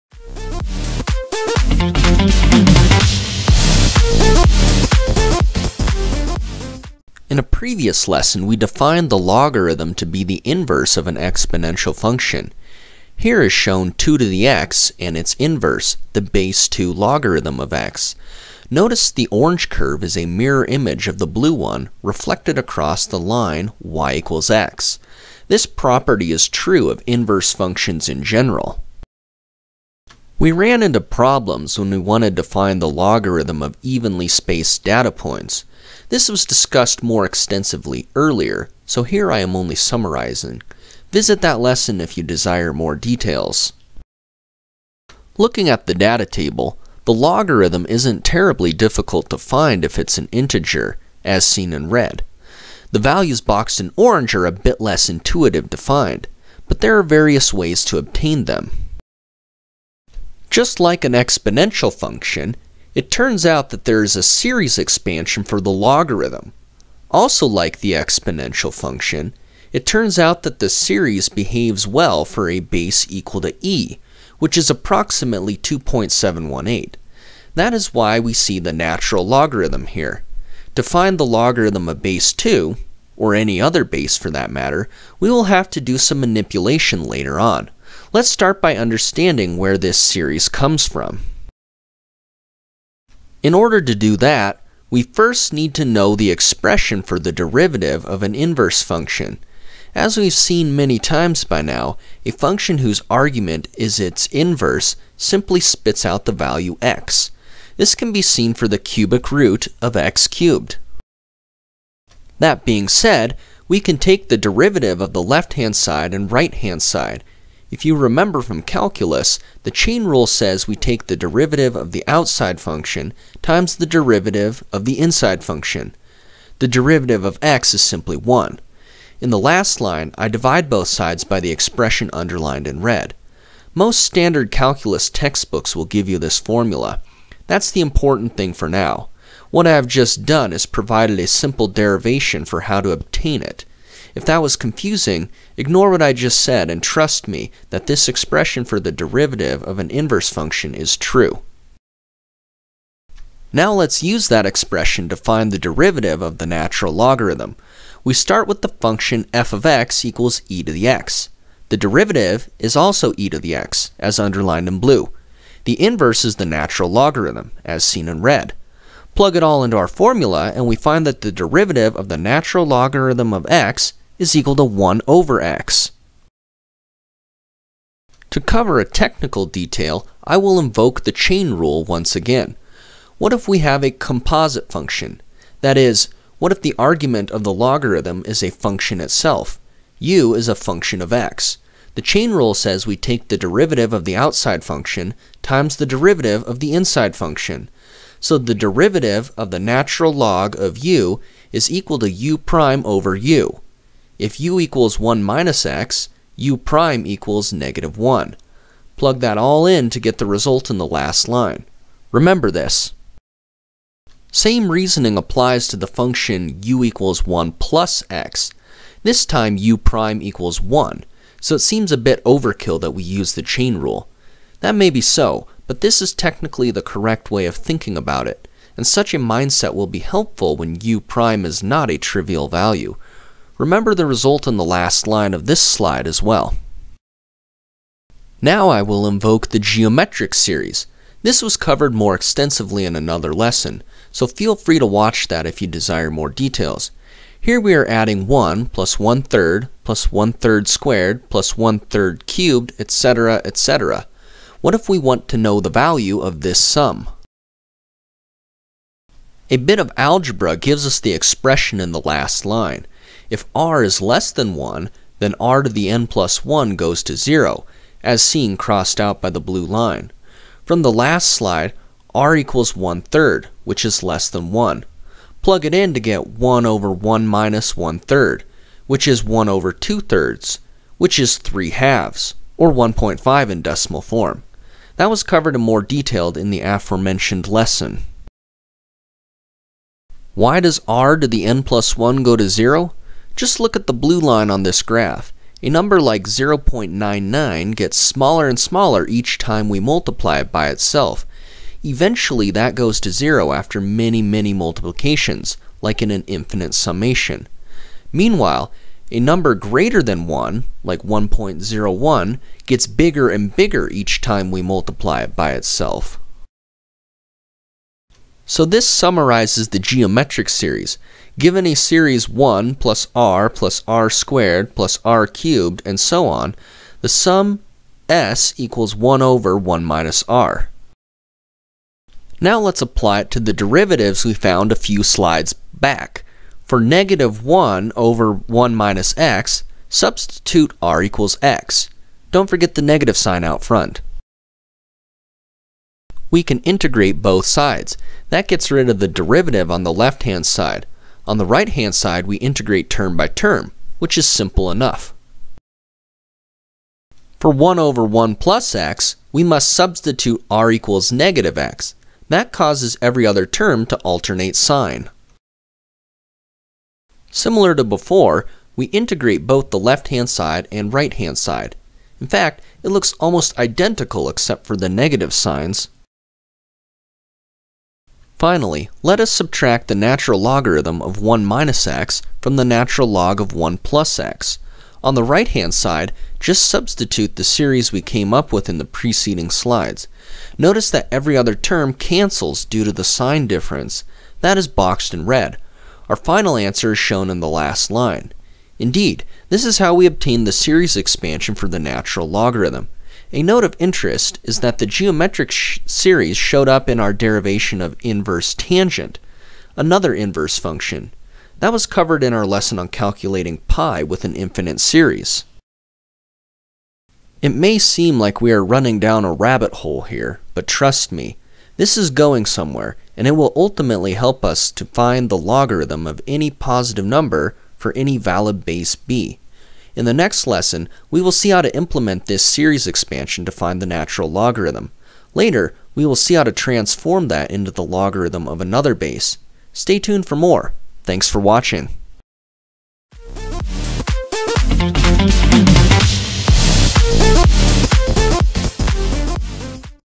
Series Expansion of the Natural Logarithm
Series Expansion of the Natural Logarithm: Now that we have defined the logarithm as the inverse of an exponential function, we explore ways to compute it. This lesson focuses on the development of the series expansion used to find the natural logarithm of a number. The derivation involves usage of the geometric series.